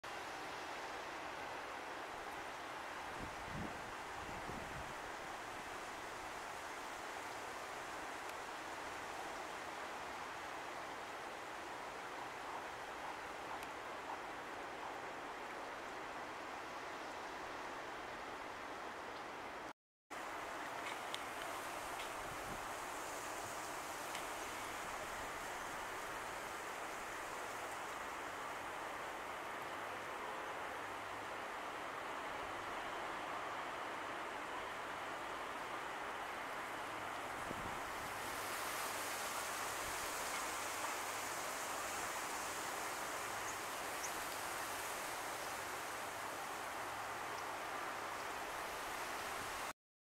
Mama Gator Yawning
She loves when I stop by for a visit